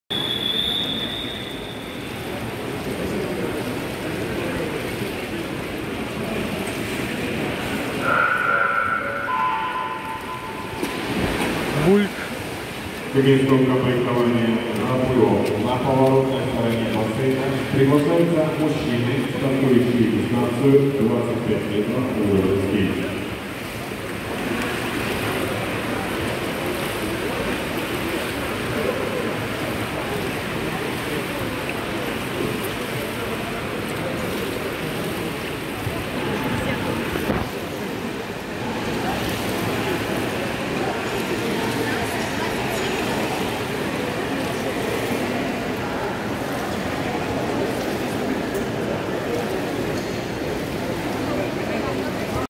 1 заплыв 25м. Вольный стиль. Женщины 60+
ГТО - ПЛАВАНИЕ
Спартакиада Санкт-Петербурга среди инструкторов Центров спорта.
Плавание проводилось 23 марта 2021 года, по адресу: Россия, Санкт-Петербург, ул.Хлопина, дом 10 Д, Спортивный комплекс "Центр плавания"

Тренируйся каждый день!